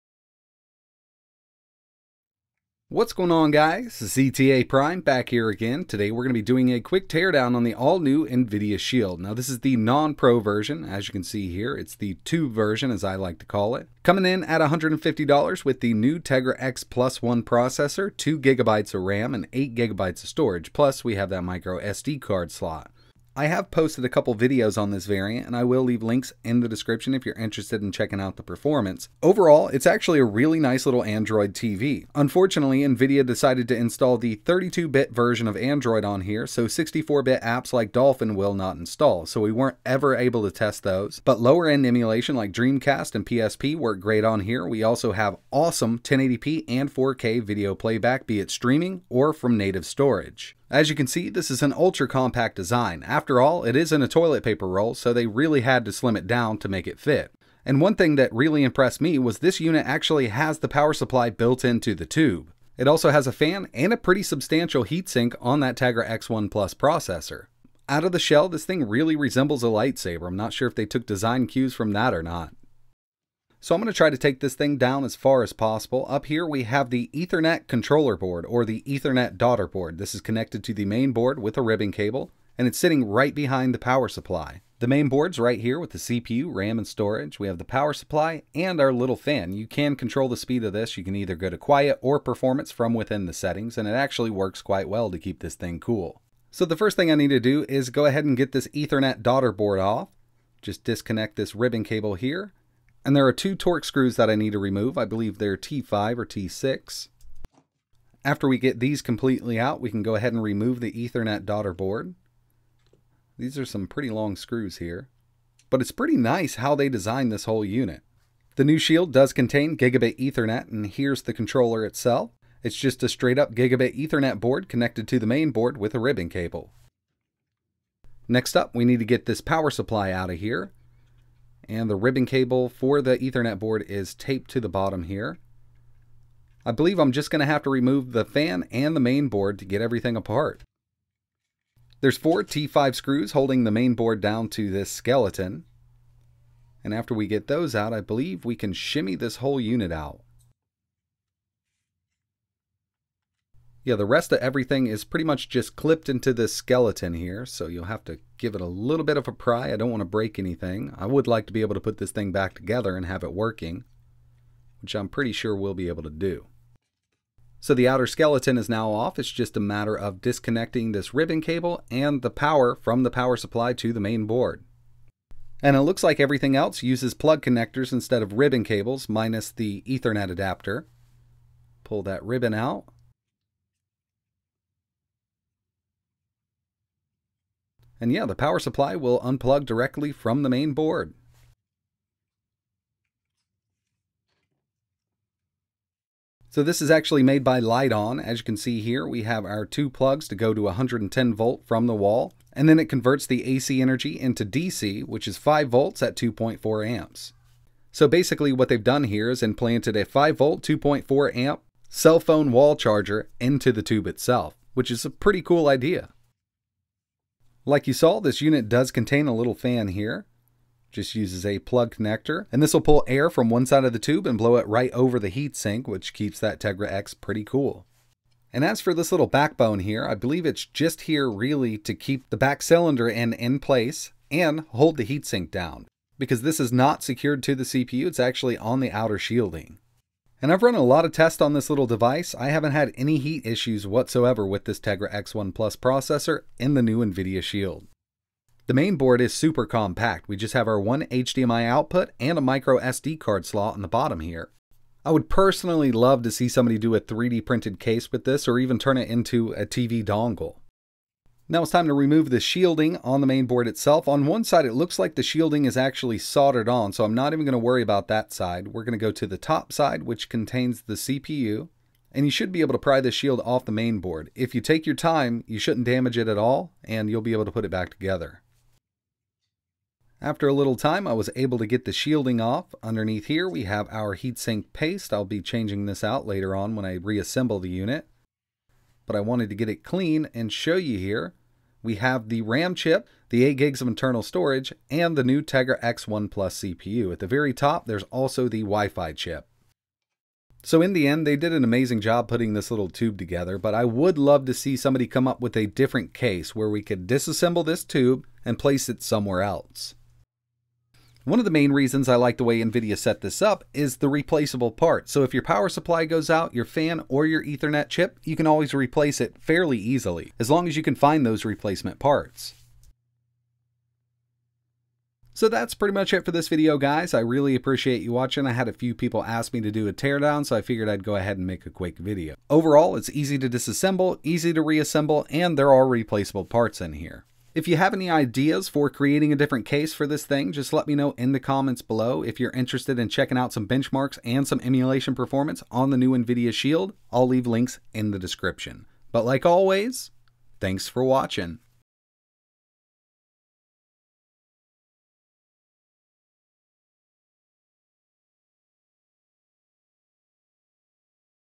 2019 Nvidia Shield TV Teardown - Tube Version
In this video, we teardown “take apart” the all-new 2019 Nvidia Shield android TV just to see how they made everything fit in a paper towhee roll! It's actually really awesome how they set this one up.
With the new Tegra x1 plus CPU 2GB of ram, 8GB of storage ethernet and built-in power supply they had to slim some comments down to make it fit.

Buy One Here:
Shield TV PRO
Shield TV